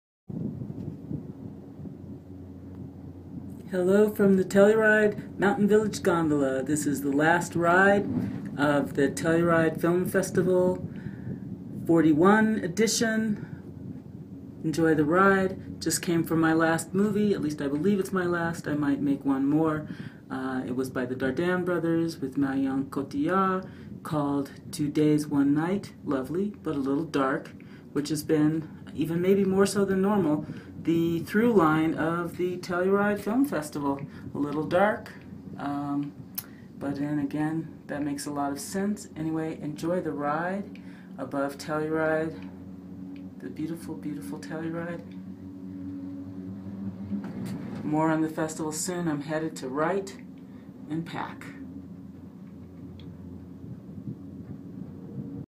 Final gondola ride of the 41st Telluride Film Festival.
By: Lisa Kennedy - Film/Theatre writer covering and discovering what takes place on stage, screen & in between. Fave goofball methodology remains Lacan and astrology